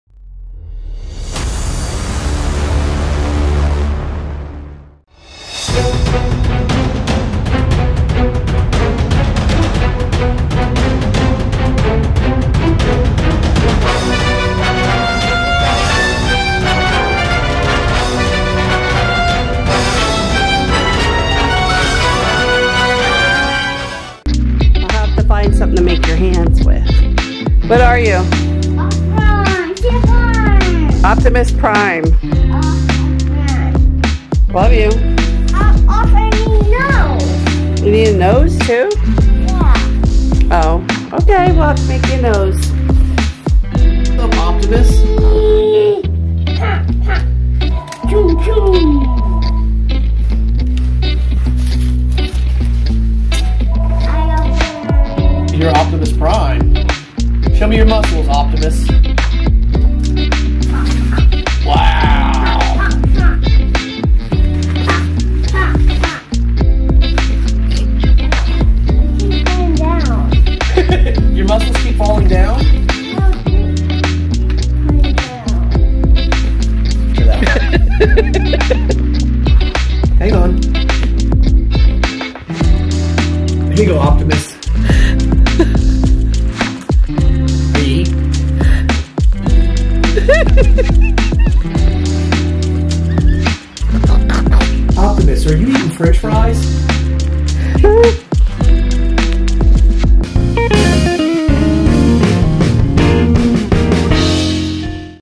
Optimus Prime | Costume build | Grandmaw's are great eatting McD's fries McDonalds
Optimus Prime Home made build with paw and grandma| Brayson's imagination is real